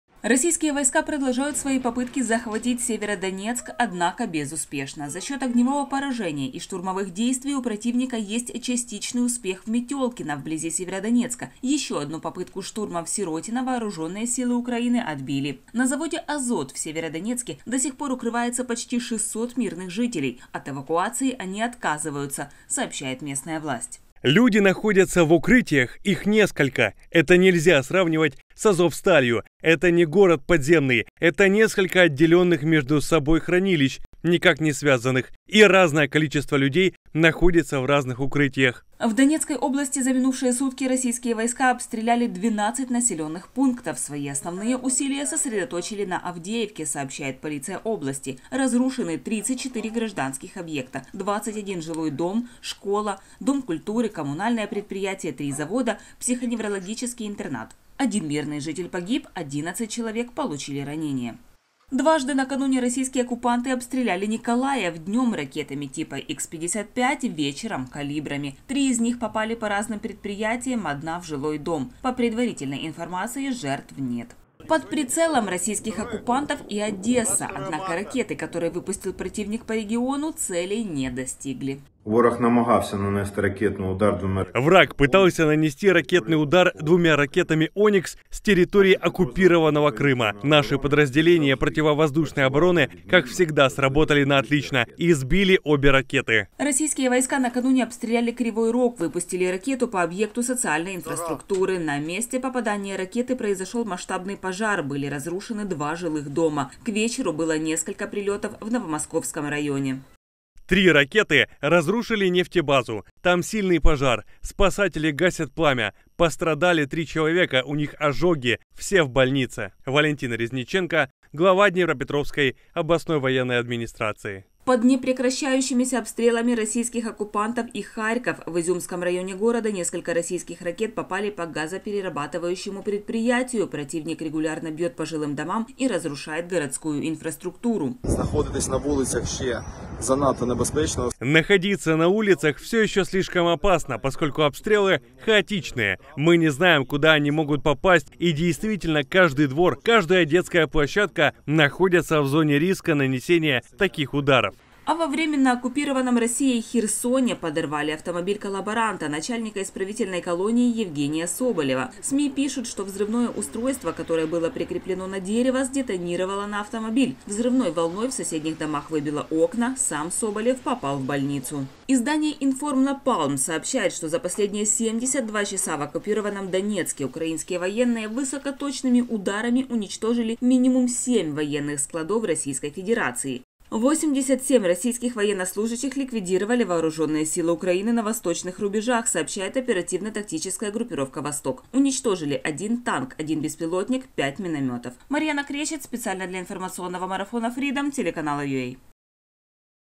Война в Украине. 116-й день. Сводка
В Донецкой области за минувшие сутки российские войска обстреляли 12 населенных пунктов. Под постоянными обстрелами живут и жители Харькова. В Изюмском районе города несколько российских ракет попали по газоперерабатывающему предприятию. Противник регулярно бьет по жилым домам и разрушает городскую инфраструктуру. Детальнее о ситуации на фронте смотрите в материале